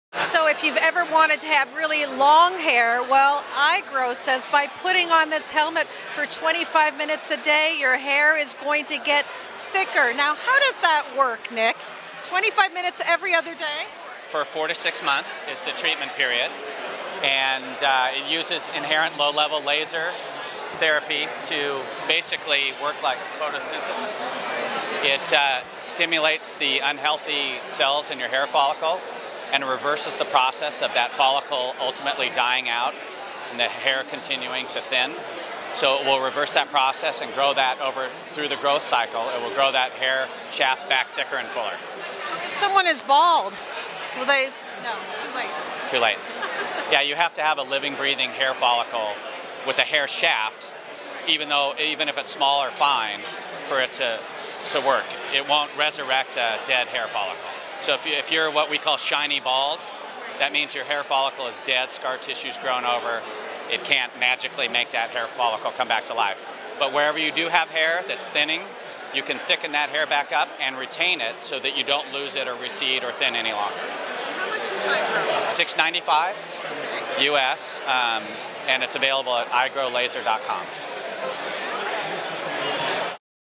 iGrow FDA Approved Laser Hair Therapy
If you ever wanted to have thicker hair.  iGrow, the FDA approved laser hair helmet says you will get just that by wearing their helmet for 25 minutes every other day for 4-6 months. It uses low level laser to work like photosynthesis.